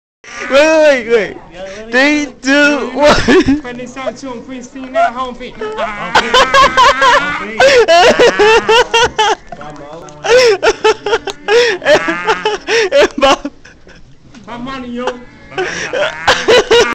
My classmate kid at James Monroe high school
This kid jesus and Jose almonte shirt off funny saying Bob Marley.